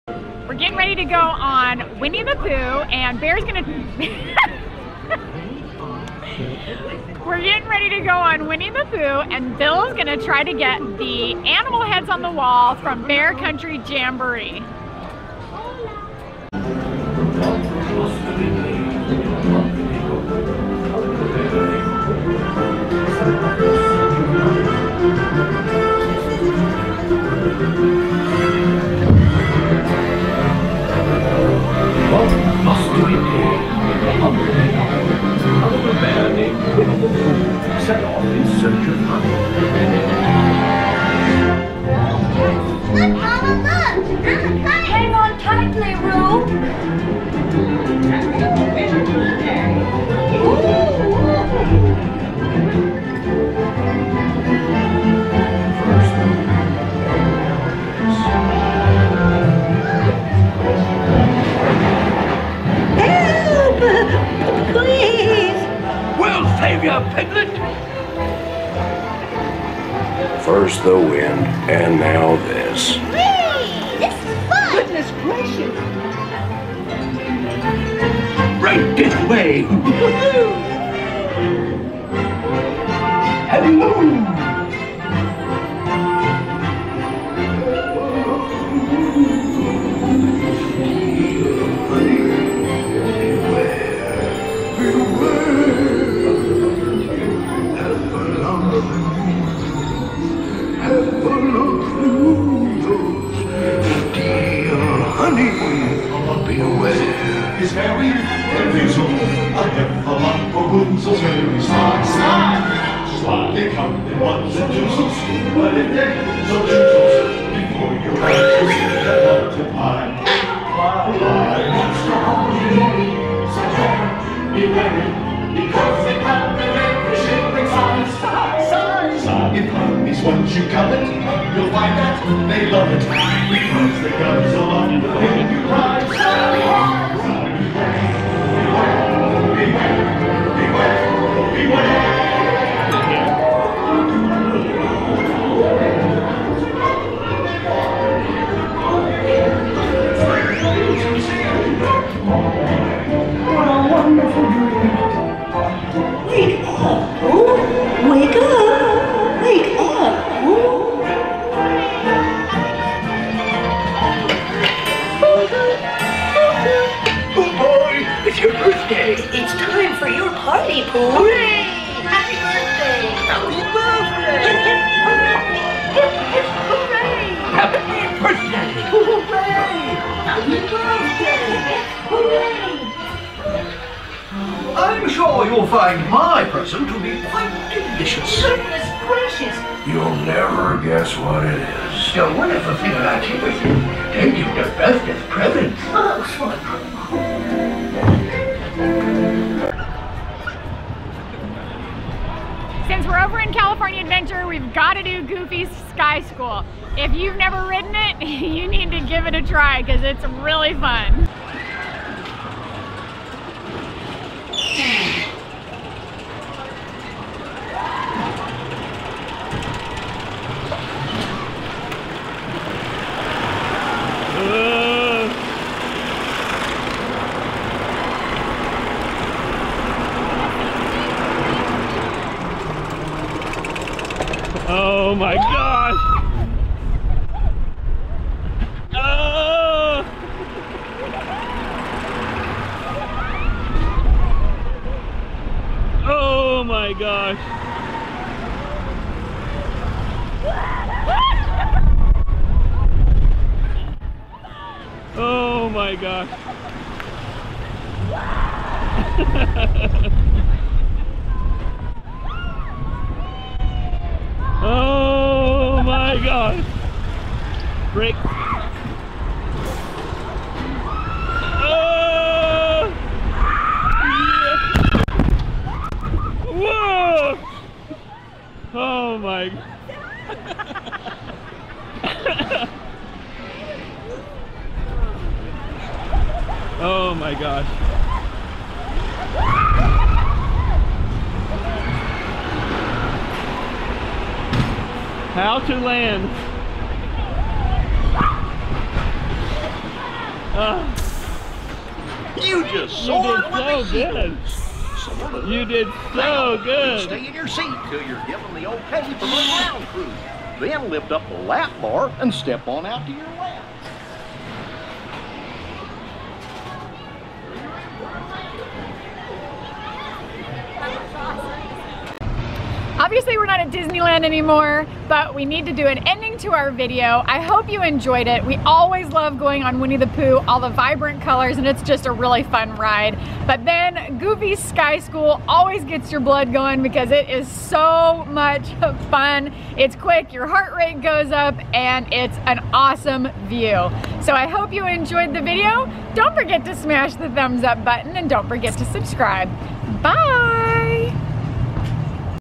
2019 Complete Rides 4k Disneyland Secrets in Many Adventures of Winnie The Pooh. Goofy's Sky School
At Disneyland California. Who remembers the Country Bear Jamboree? Remember the talking animal heads on the wall. They are still there inside the Many Adventures of Winnie The Pooh ride. Bill has finally taken proper video showing the heads. Not only do you need to know how and where to look but to video them in their dark hiding place is a very hard thing to do. Also, If you have never been on Goofy's Sky School in California Adventure, Do not be scared. Just do it. It is a blast. Have an awesome day, Billisa